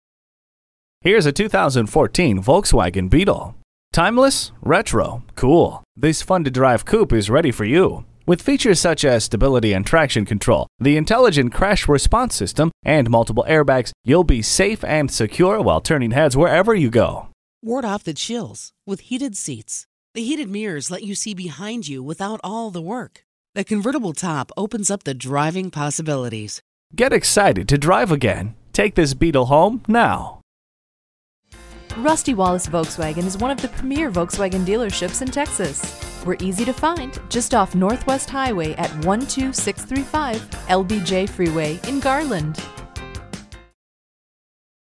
2014 Volkswagen Beetle Dallas TX Garland, TX V140721 - SOLD
Year: 2014
Make: Volkswagen
Model: Beetle
Trim: 1.8T
Engine: 4 Cyl. 1.8 L
Transmission: Automatic
Color: Reflex Silver Metallic/Black Roof
Interior: Titanium Black Leatherette
Stock #: V140721
VIN: 3VW507AT2EM819875
Turbocharged! Hey! Look right here! Don't miss out on purchasing this good-looking 2014 Volkswagen Beetle. It will take you where you need to go every time...all you have to do is steer! Come talk to us for the best deal.

Address:
12635 LBJ Freeway